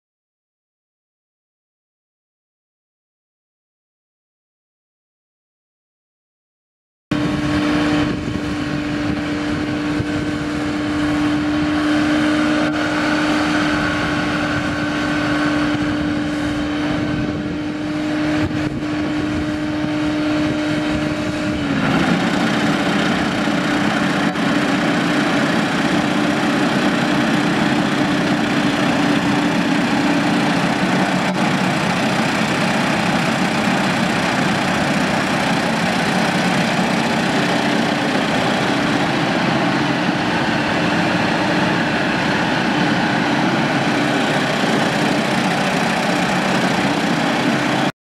1995 New Holland 2550 swather for sale | sold at auction September 10, 2014
1995 New Holland 2550 swather
1,209 hours on meter
Approx. 5,000 total hours
Four four cylinder diesel engine
110 HP
Hydrostatic transmission
AC and heat
AC blows cold
Air suspension seat
2300 series platform
14&#039;W
Rubber conditioner rollers
16.9-24 front tires
9.5L-15 rear tires
Left side final drive replaced in summer 2013
Right side hydraulic drive motor replaced two years ago
Head lift pump replaced in summer 2014
Air leak in right front tire
Small hydraulic leak on left side drive motor
Serial 567886

It was located in Leavenworth , KS, and we sold it to the highest bidder for $17,050.